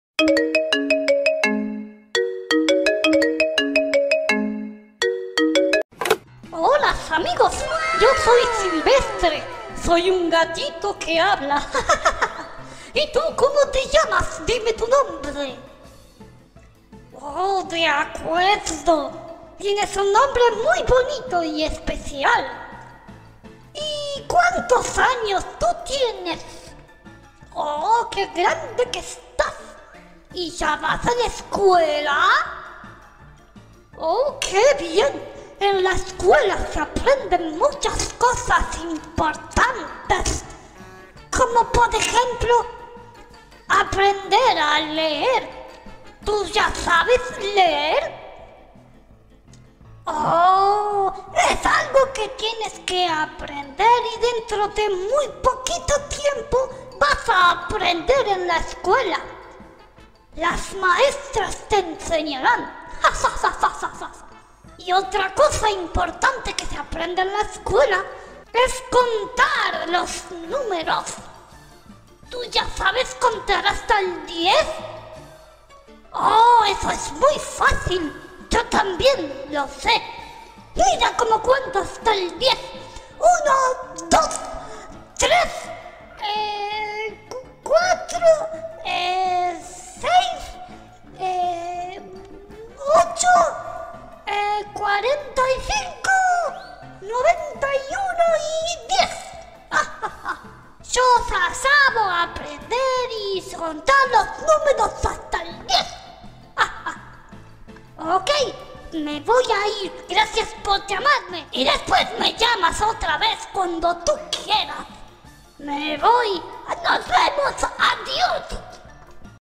Llamada a SILVESTRE - Silvester - Gatito - CAT
Llamada a Silvestre, el gato! Espero que disfruten de la conversación, cuyo fin es entretener a todos!
Muchas gracias por darnos esta oportunidad!